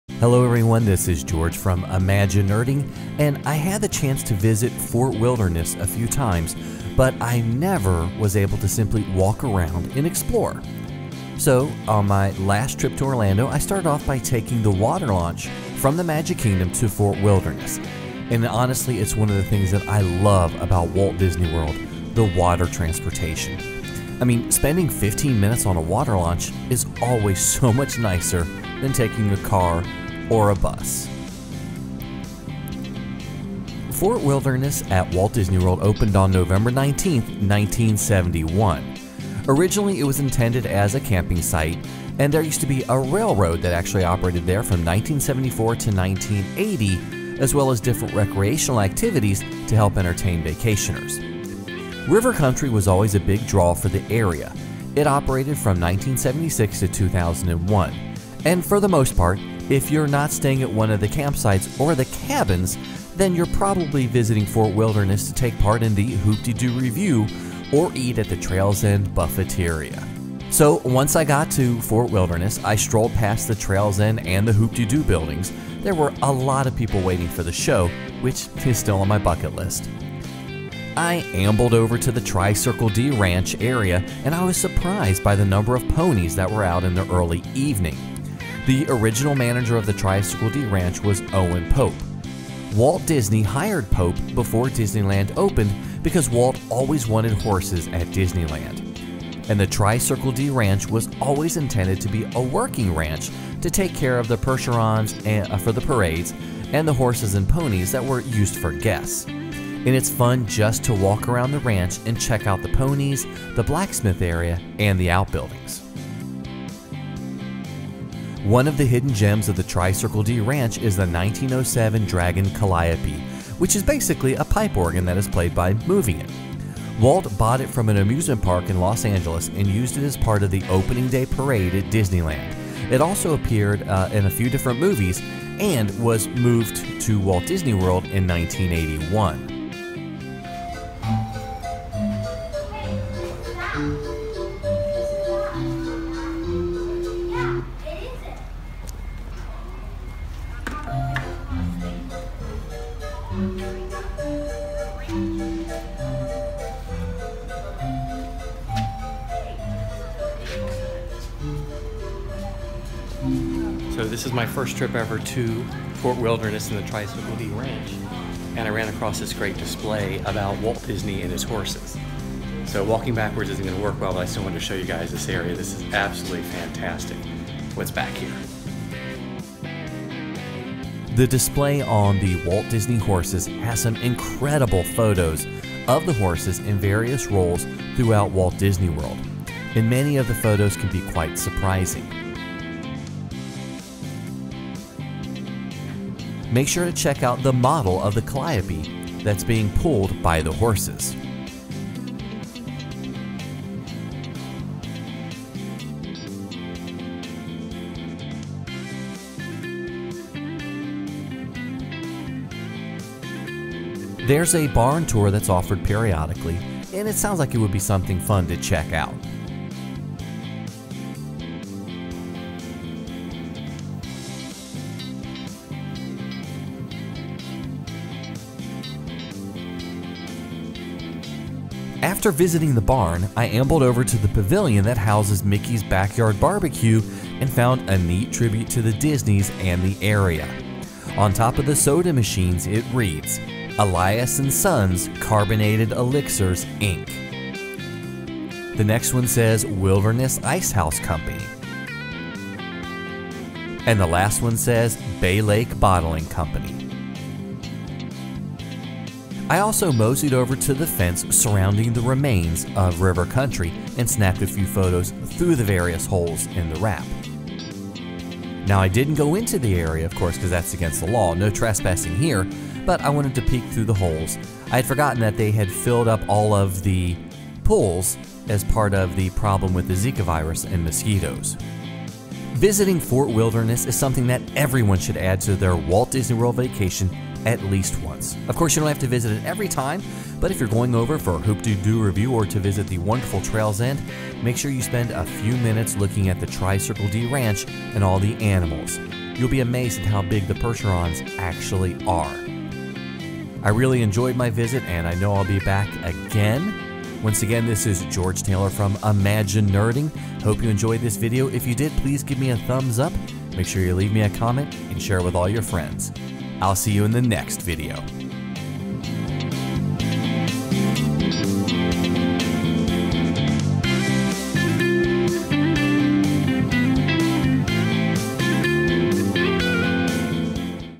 Tri-Circle-D Ranch and Farm & River Country at Fort Wilderness at Walt Disney World
Talk about hidden gems of Walt Disney World: George explores the Tri-Circle-D Ranch and Farm at Fort Wilderness. He checks out the ponies, percheron and people!

George shares video of the 1907 Dragon Calliope that Walt Disney purchased before Disneyland opened. He also checks out the wonderful display on Walt Disney and Horses.

As a special bonus, he sneaks a few photos of the abandoned River Country!

I ambled over to the Tri-Circle-D Ranch area and I was surprised by the number of ponies that were out in the early evening. The original manager of the Tri-Circle-D Ranch was Owen Pope. Walt hired Pope before Disneyland opened, because Walt always wanted horses at Disneyland. The Trip-Circle-D Ranch was always intended to be a working ranch to take care of the Percherons for the parades, and the horses and ponies used for guests. It's fun to just walk around the ranch and check out the ponies, blacksmith areas and outbuildings.

One of the hidden gems of the Trip-Circle-D-Ranch is the 1907 Dragon Calliope, which is basically a pipe organ that is played by moving it. Walt bought it from an amusement park in Los Angeles and used it as part of the opening-day parade at Disneyland. It also appeared in a few movies and was moved to Walt Disney World in 1981.